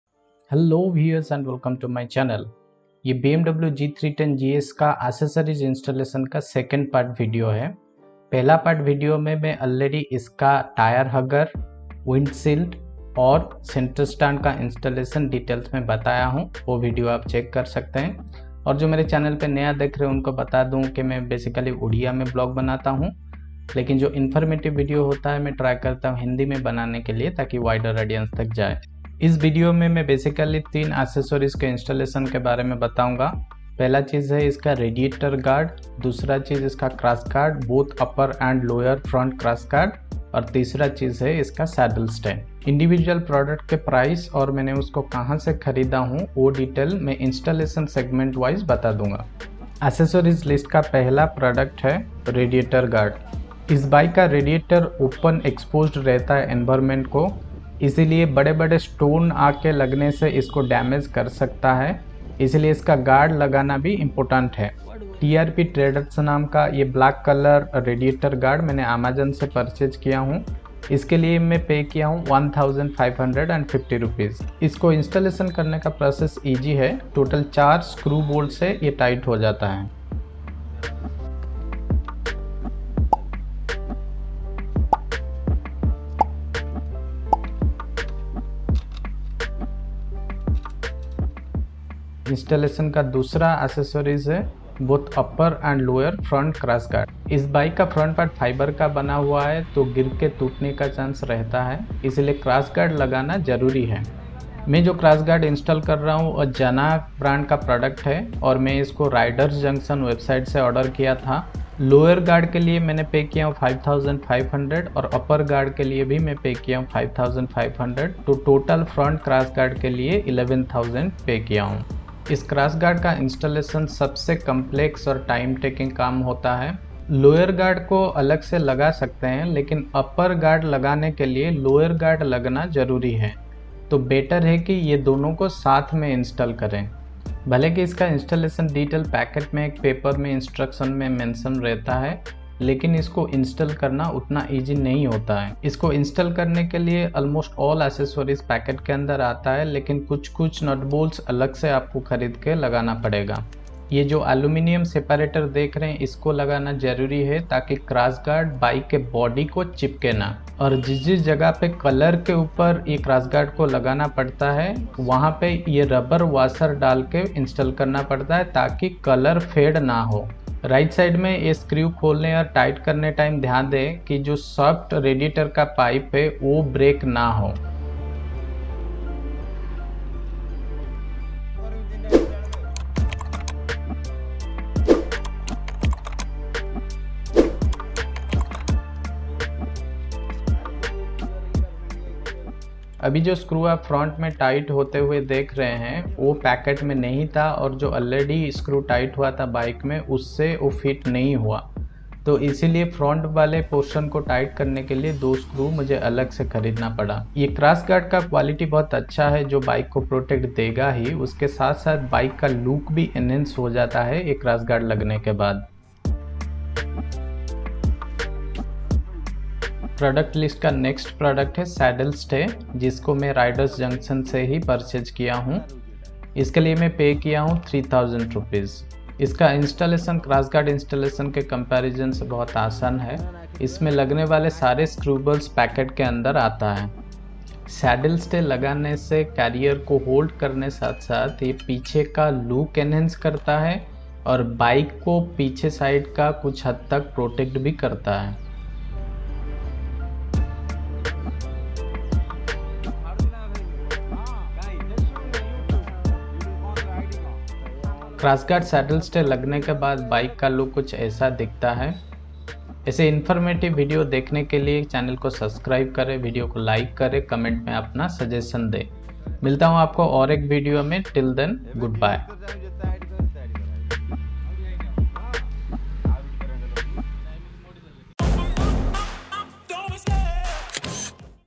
Crash guards, Radiator guard, Saddle Stay installed on BMWg310GS | Buying description in the Video
This is 2nd part of accessories installation on BMW g 310 GS.

Tried to explain the product and installation details. If you have any doubt, then please put those in the comment section. I'll be happy to answer.

Product details link mentioned below.

Radiator Guard -

Let me know on which specific topic you need video, I'll create for same.